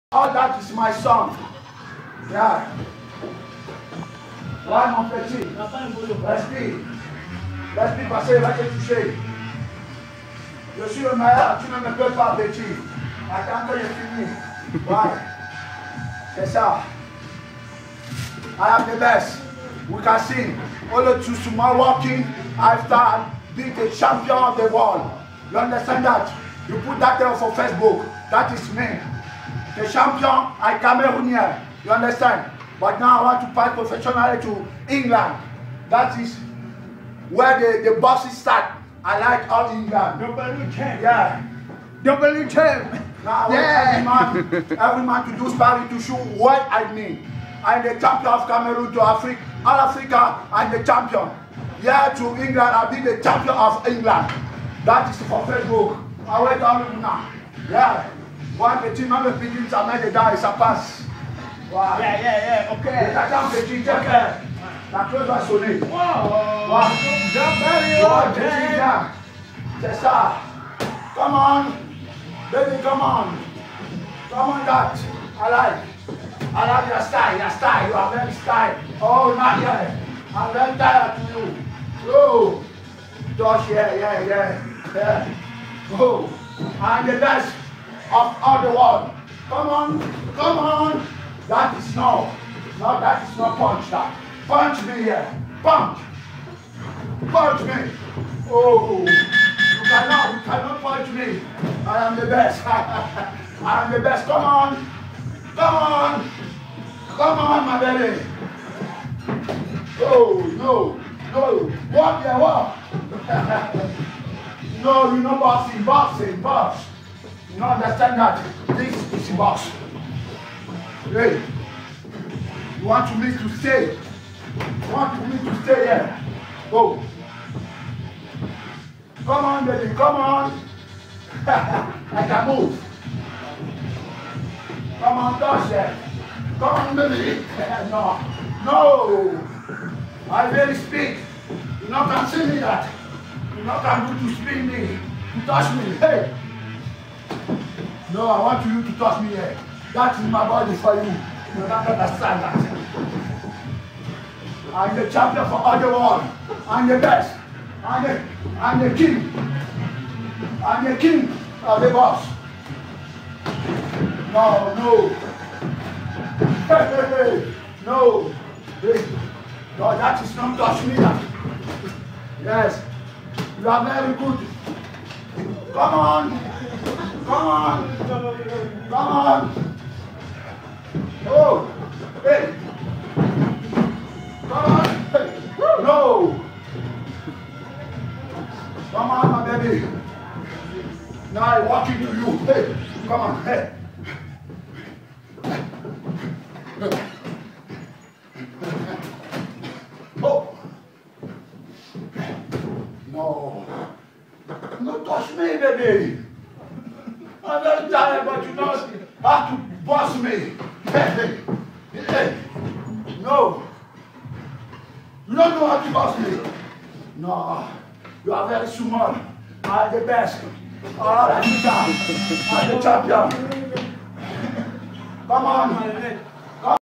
DOUBLE JAB A.B.C. CAMEROONIANS BLAISE MENDOUO V THOMAS ESSOMBA SPARRING
Blaise gives an Ali style rant before a complete mismatch with little Thomas Essomba in one of the funniest sparring sessions I've seen in a long time!!